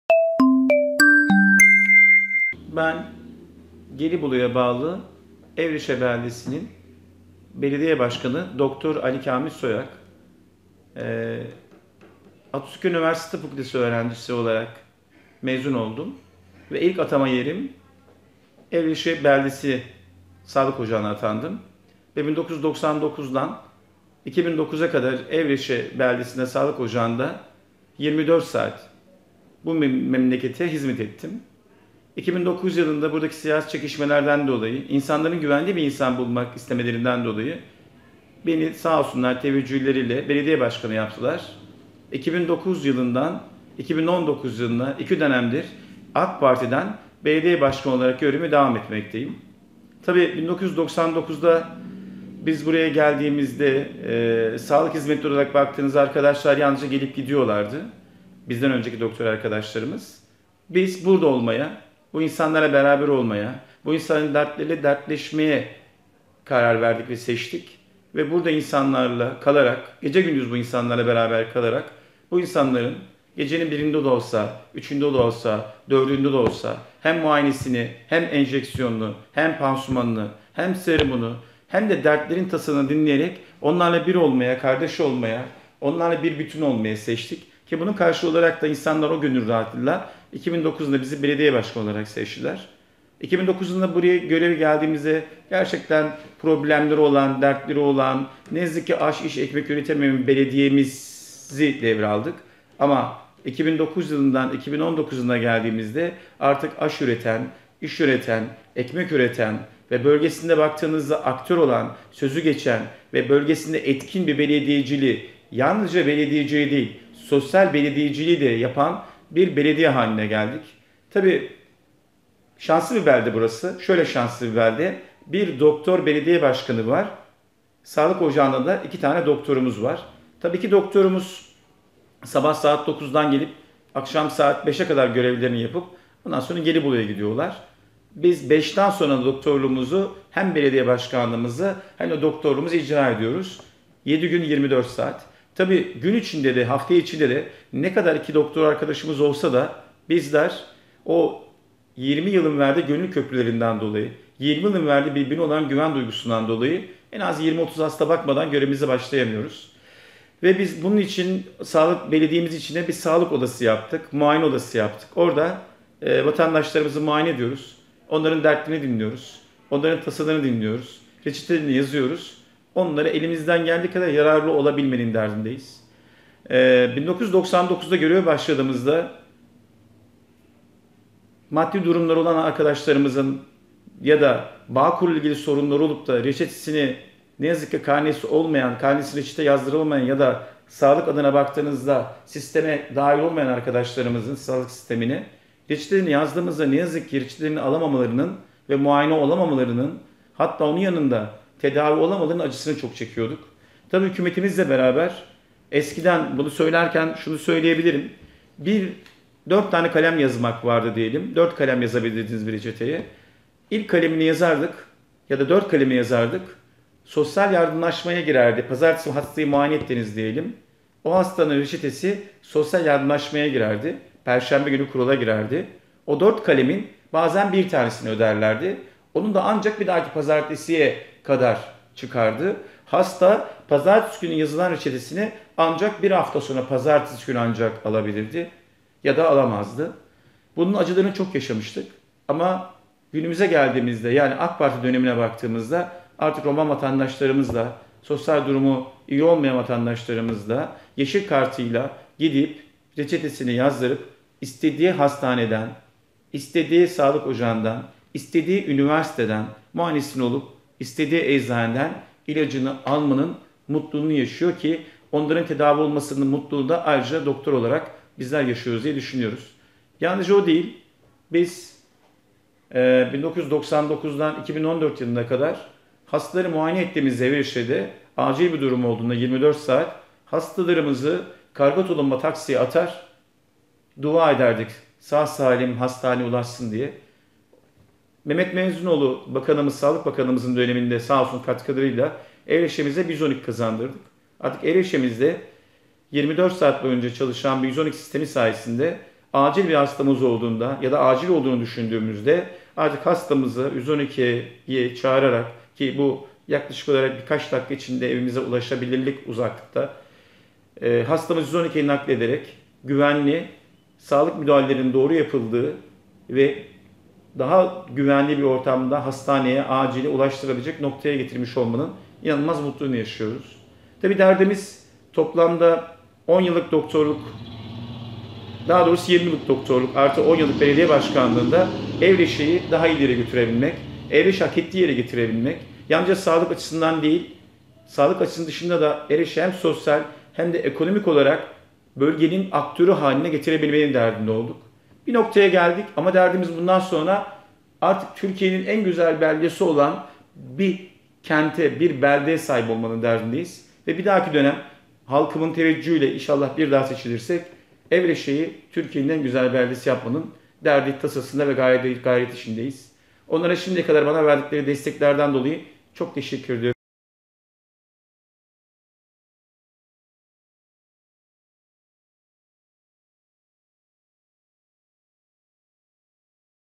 Evreşe'nin doktor belediye başkanı Ali Kamil Soyuak
Çanakkale'nin Evreşe Belde Belediye Başkanı Dr. Ali Kamil Soyuak, güne makamında hasta muayene ederek başlıyor. Hemen ardından belediye hizmetine yöneliyor. 1999'da ilk görev yeri olarak Evreşe'ye atanan Soyuak 10 yıllık doktorluk hizmetinin ardından 2009'da belediye başkanı seçildi. Şimdi ise hem doktorluk hem de belediye başkanlığı görevini sürdürüyor.